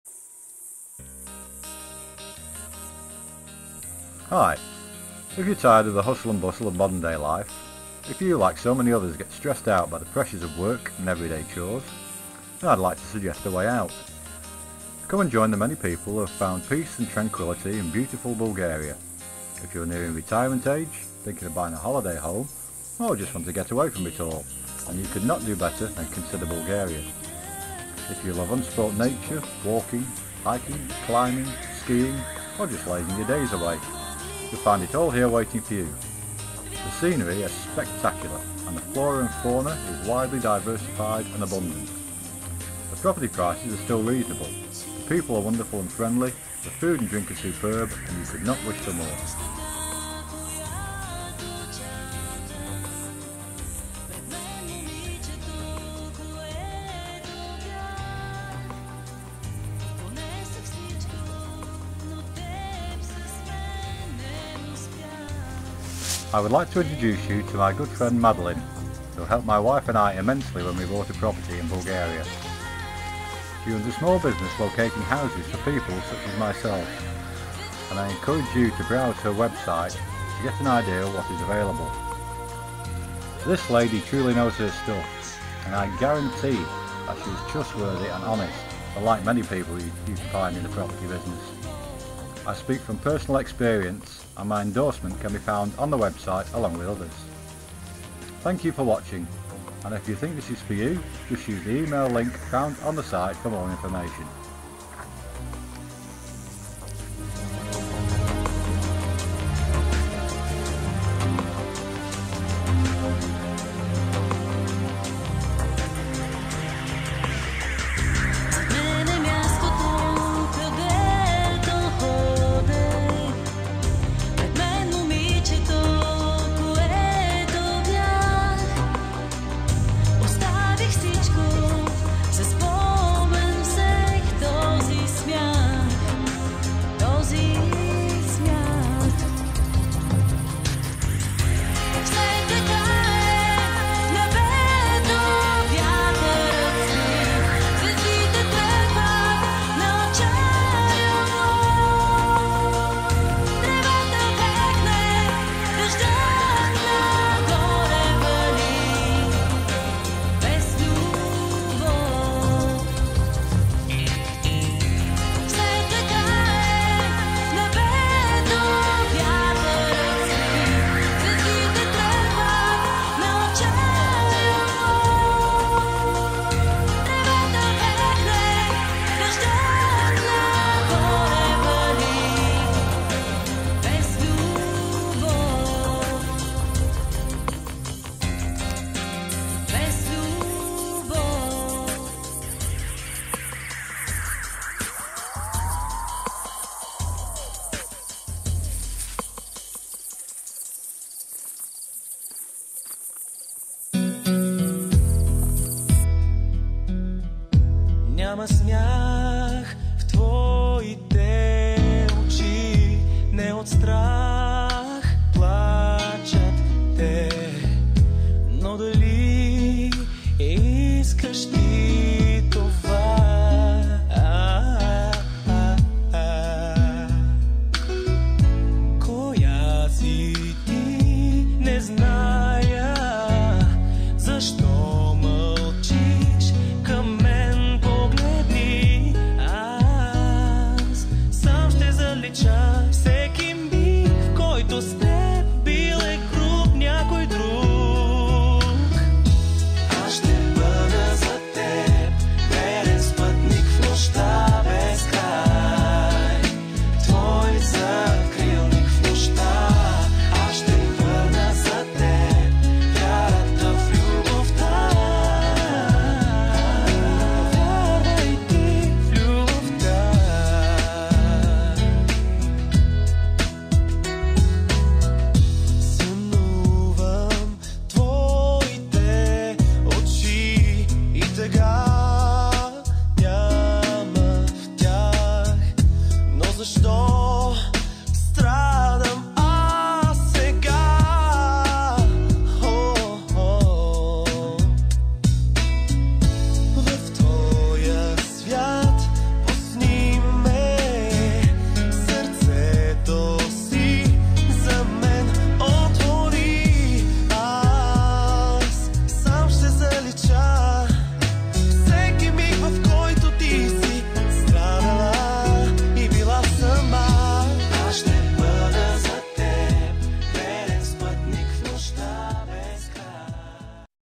A NEW LIFE IN BULGARIA
A video view of unspoilt Bulgaria, where the pace of life is slow and peaceful, showing you the prospect of a better quality of living.